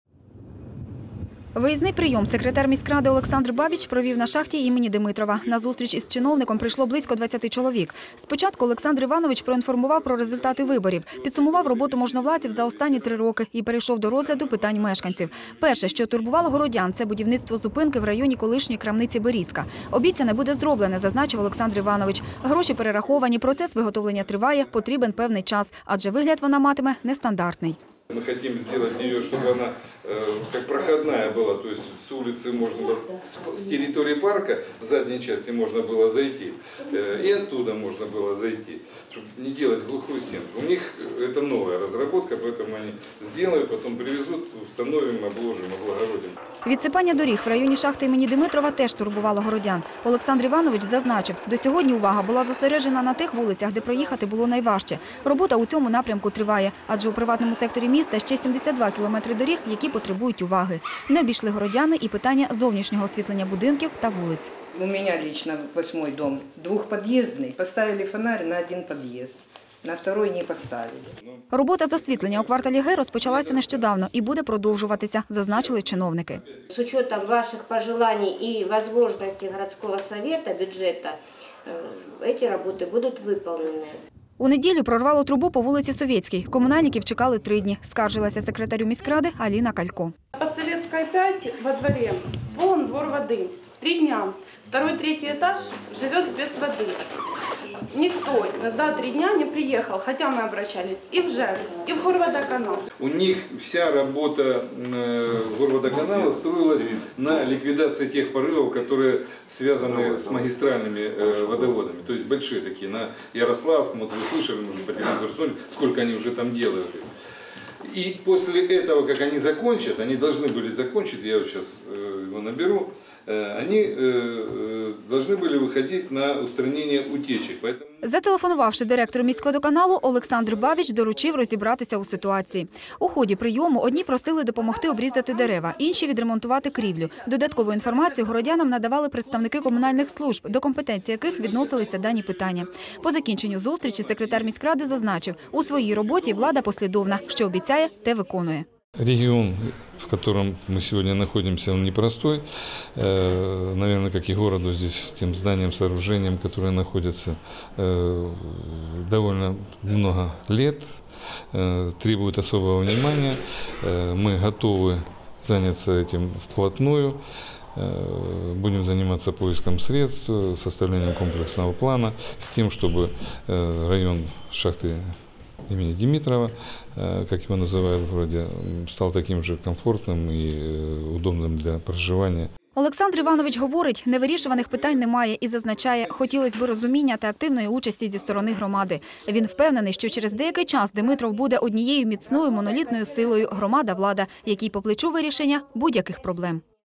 Секретарь горсовета встретился с жителями
В последнее время в адрес исполнительного комитета Димитровского городского совета и непосредственно секретаря Александра Бабича поступило несколько обращений от горожан, проживающих в районе шахты имени Димитрова. Касались они состояния дорог, освещения, отопления домов и других коммунальных проблем. Чтобы не терять времени, секретарь горсовета решил разобраться во всем на месте и встретился с жителями данного района в зале шахты им. Димитрова.